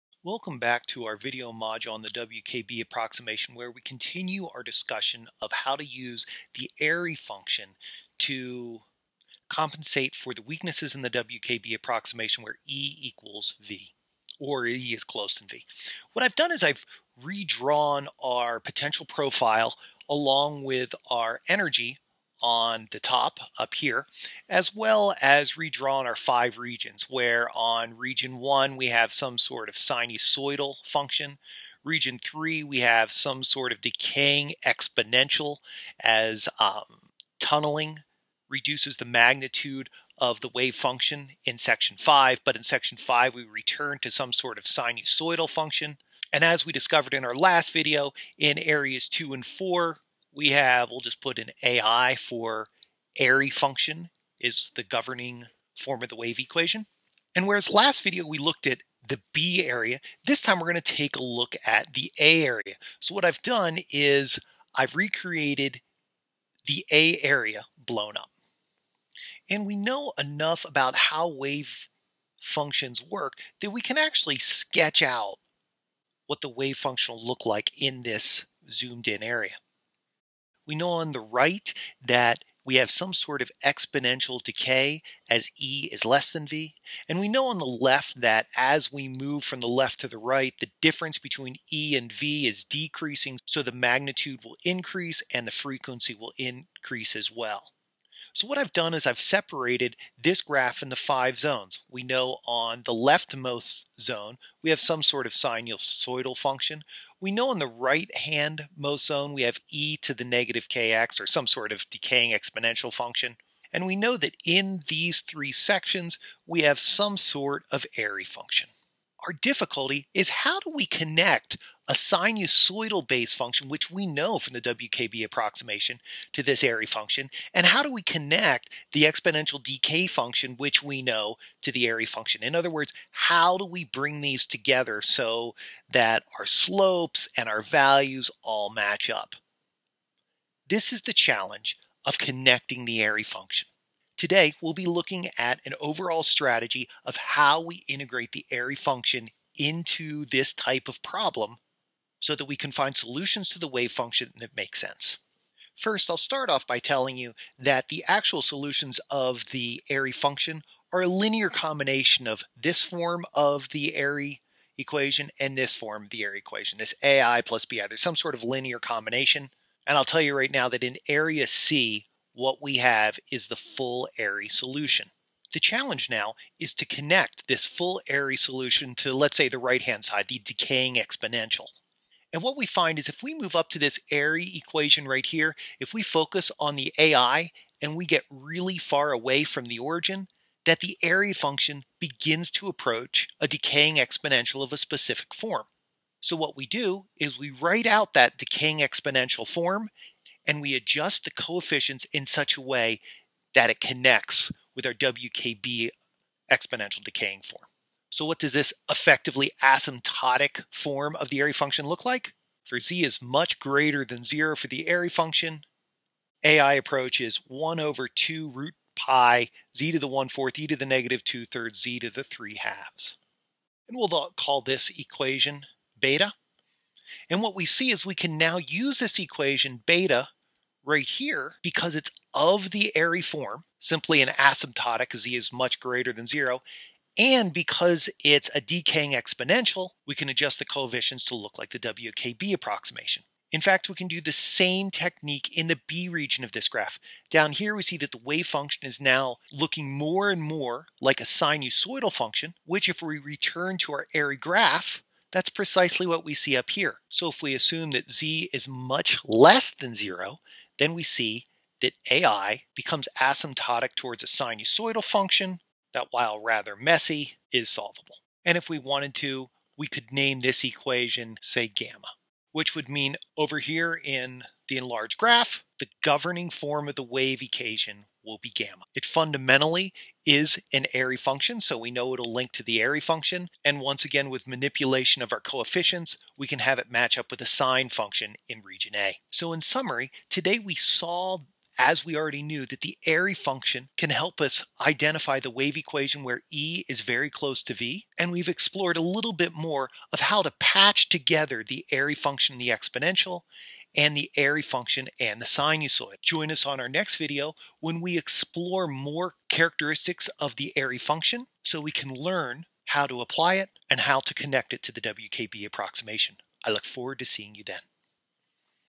QM 8.1-05 WKB Approximation, Airy function continued (5:19)
This video is part of a series on quantum mechanics that explores the WKB Approximation.  Please note that this video CONTINUES the argument developed in 8.1-04, and should be watched after it.  Here, we explore the strategy of using the Airy patch.  The content is rooted in the Griffiths text.